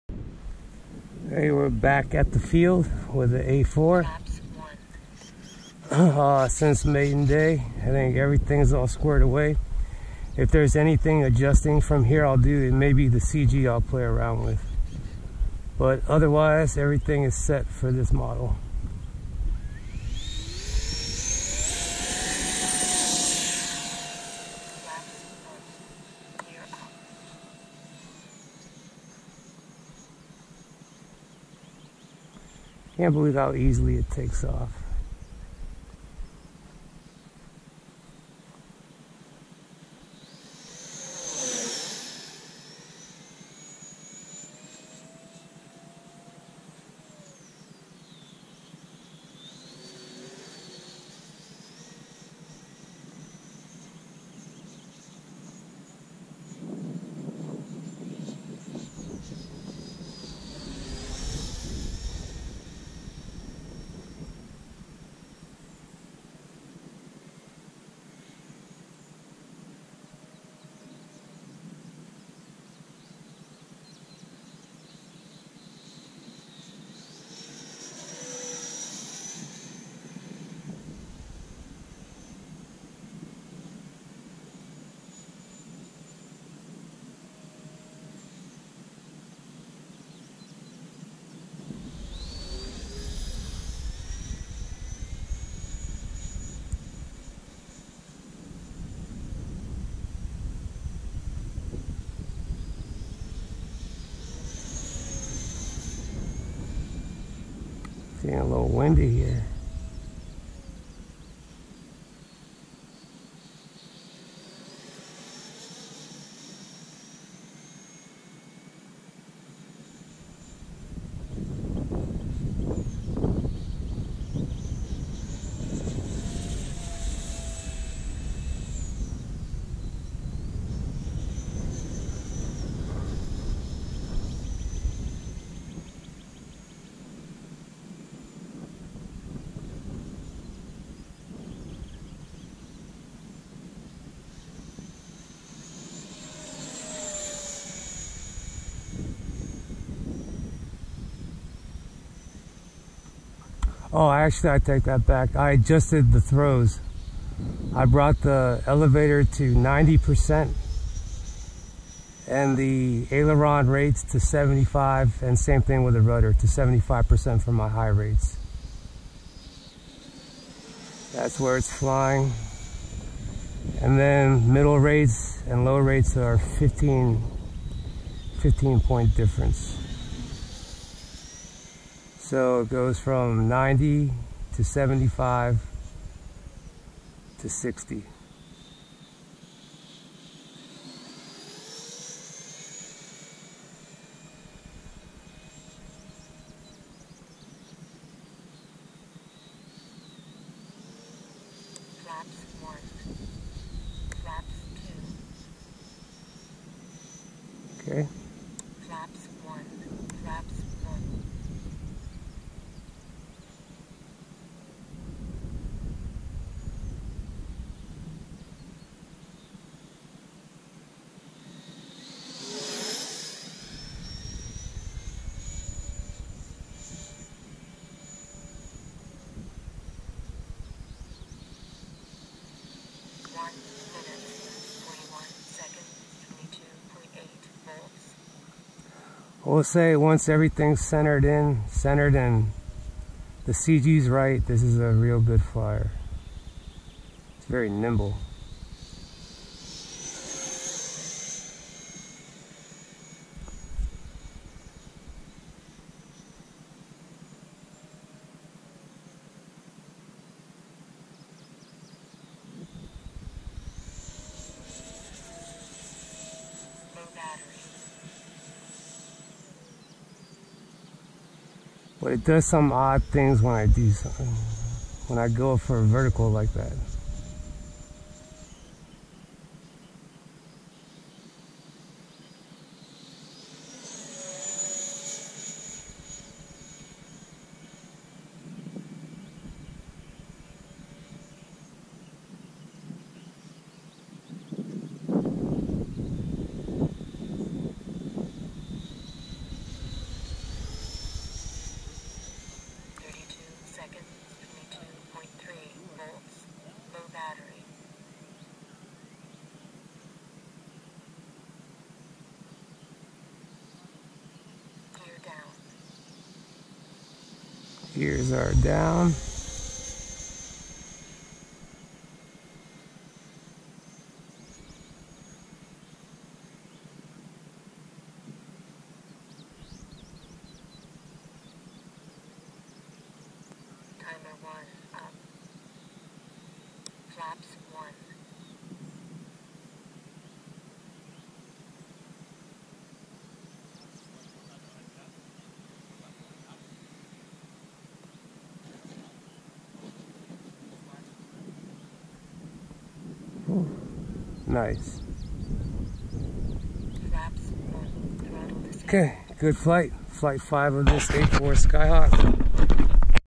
Freewing A-4E/F Skyhawk 80mm Flt 5 Squared away! Adjusted throws! Odd things going vertical! Nice!
Freewing A-4E/F Skyhawk High Performance 80mm

Radio/Receiver: RadioMaster TX16S (Carbon), Upgrade parts set (Silver), Leather Side Grips (Black), TX16S CNC AG01 Hall Gimbals / FrSky RX6R 2.4GHz ACCST Receiver and FrSky MLVSS

Battery: SMC HCL-HP 22.2V-6000mAh-80C (6s)

- Upgraded Elevator servo to the Hitec HS-5085MG Digital Metal Gear Micro Servo
- Replaced spring in the nose gear with fuel tubing
- Turned the tailhook and refueling probe removable by securing the items with a small servo screw

6/21/2024